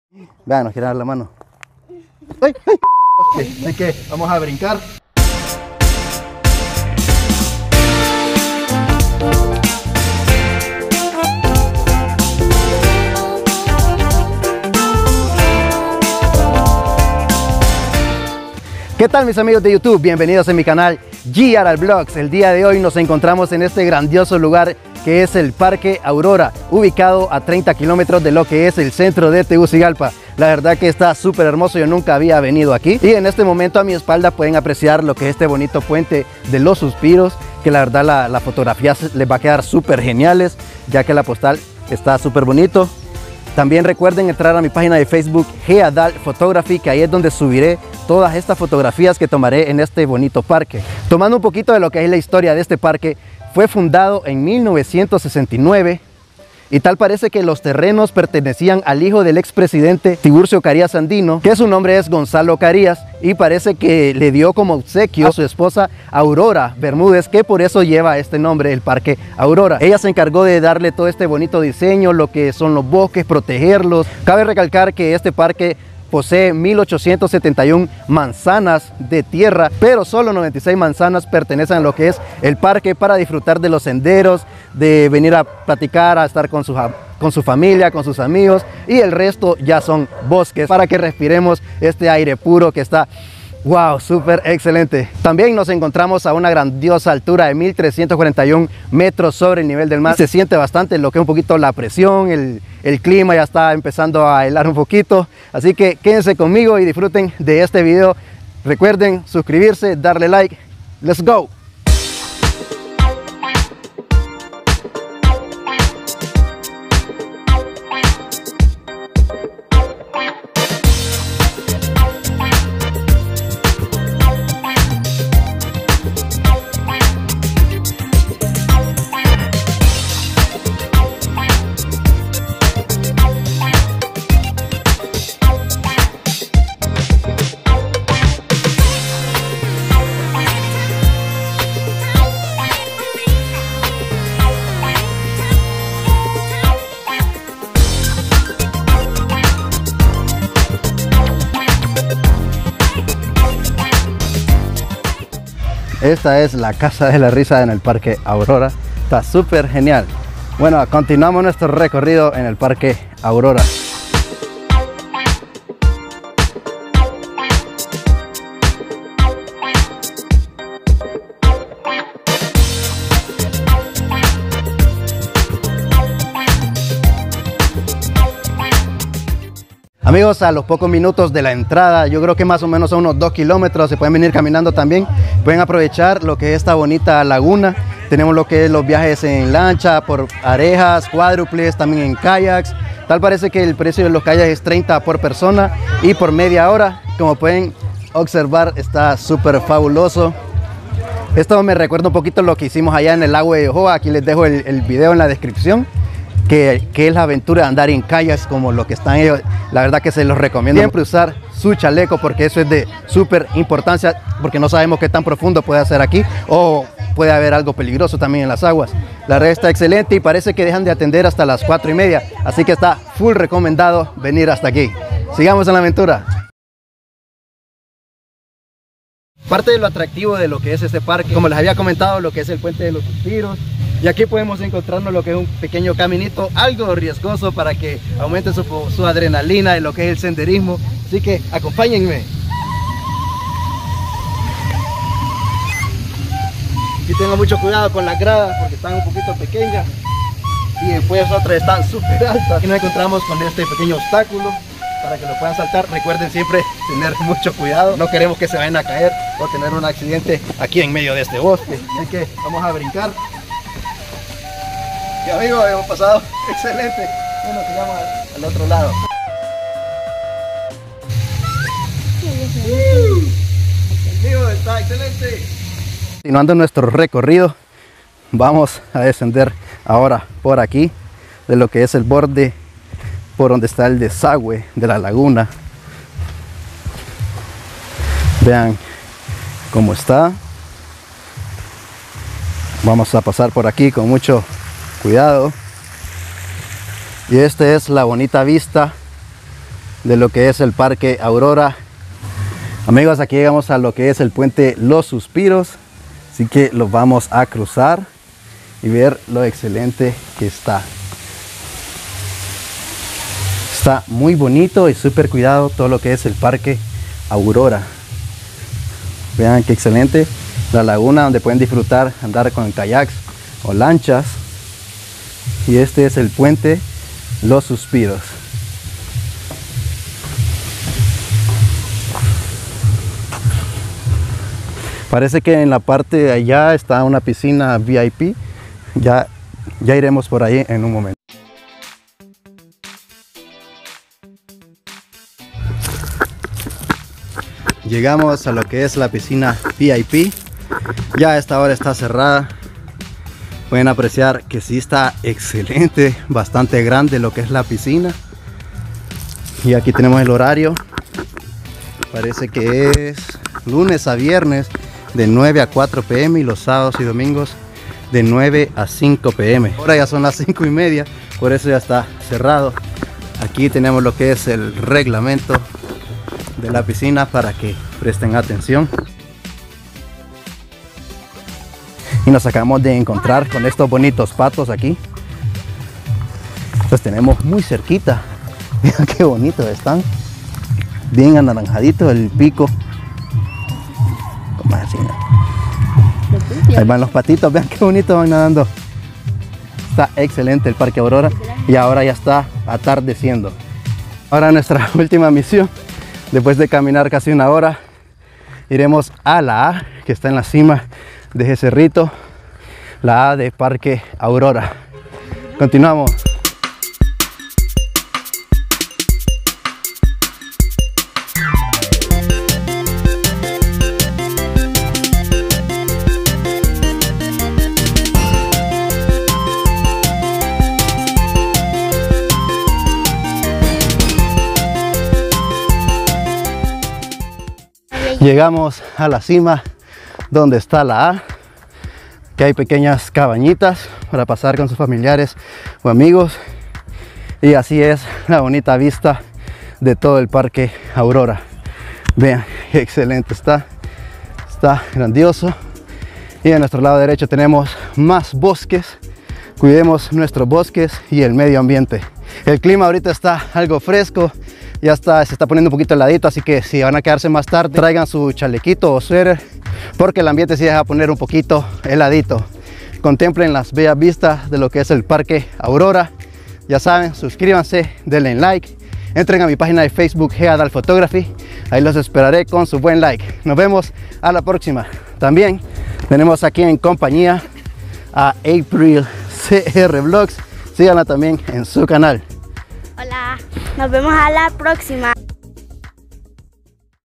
Parque Aurora | Zambrano F.M | Honduras 🇭🇳
Guión

00:00 Intro
00:17 Datos interesantes
01:50 Pequeño zoológico
02:29 Casa de la risa
02:42 Recorriendo el parque Aurora
02:58 Lanchitas y kayaks
03:46 Puente de los suspiros
05:35 Piscina VIP
06:19 Laguna
06:44 Cima del cerro
07:41 Despedida